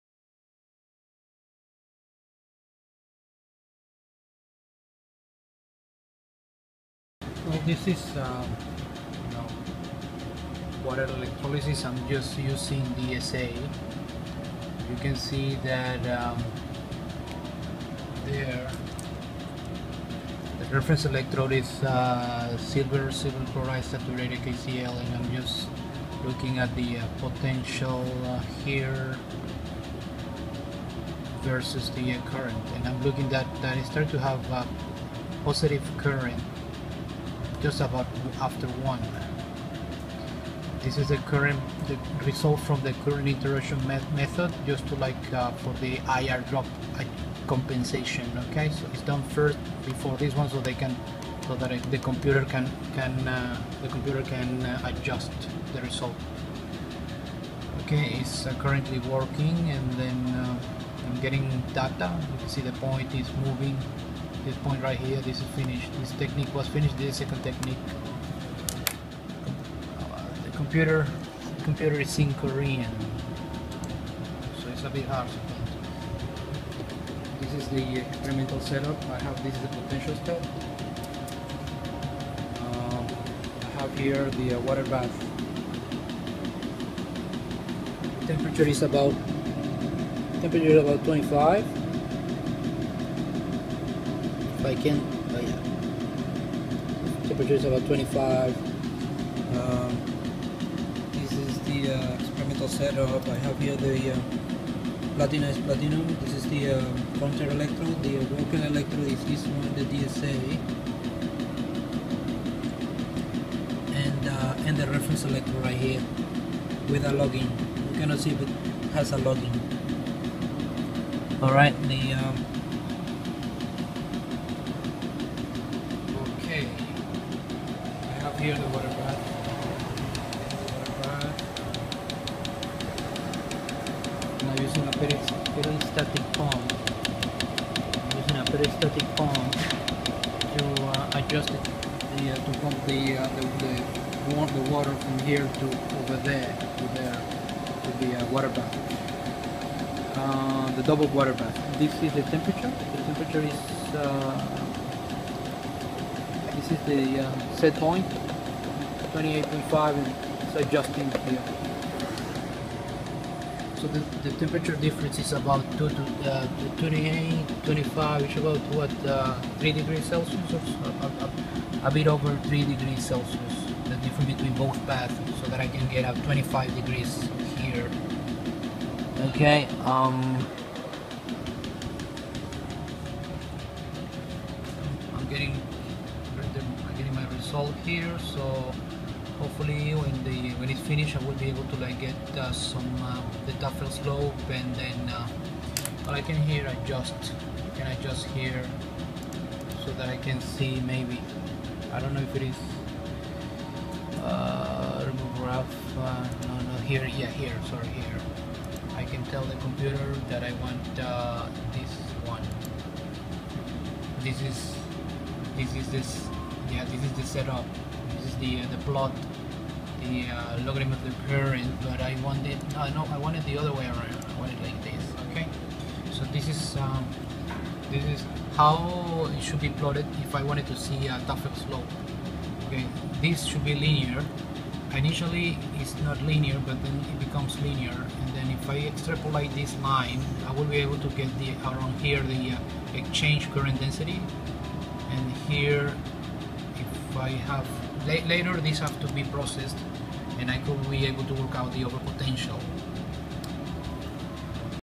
Linear Voltammetry Oxygen evolution
Linear voltammetry for oxygen evolution on DSA or IrO2 in 2 M H2SO4.